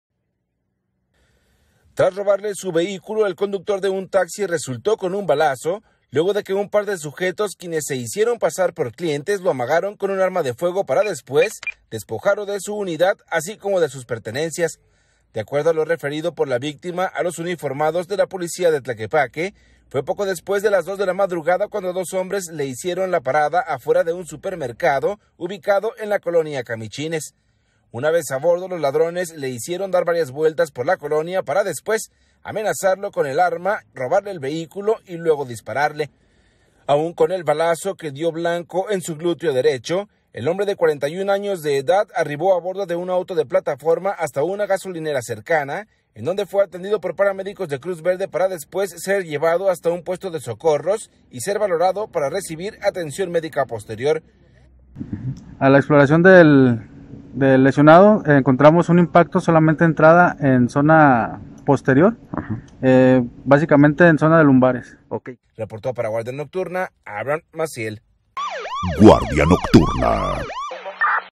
Dos asaltantes dispararon y despojaron de su automóvil a un taxista en la colonia Camichines
Anoche, en la colonia Camichines de Tlaquepaque, dos asaltantes se hicieron pasar por simples clientes para robarle su automóvil a un taxista, quien intentó defenderse del robo y terminó recibiendo un disparo